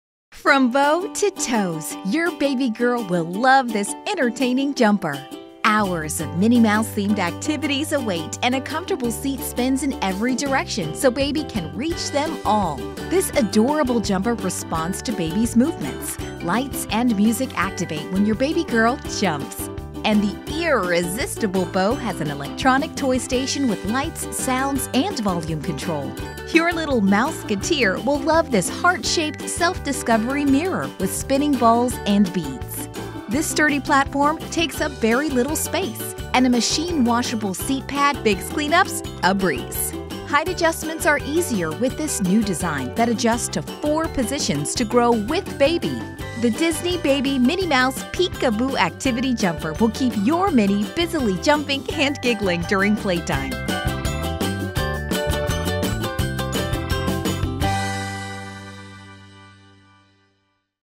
Minnie Mouse PeekABoo Activity Jumper
The Minnie Mouse Peek-a-Boo Activity Jumper is EAR-resistibly fun for your little one. Four easy-adjusting height options that grows with baby, and a seat that rotates in all directions will give baby a BOW-tastic time. With more than 12 engaging toys and activities, your baby girl will jump and explore all afternoon. Learn about all of the fun and adorable features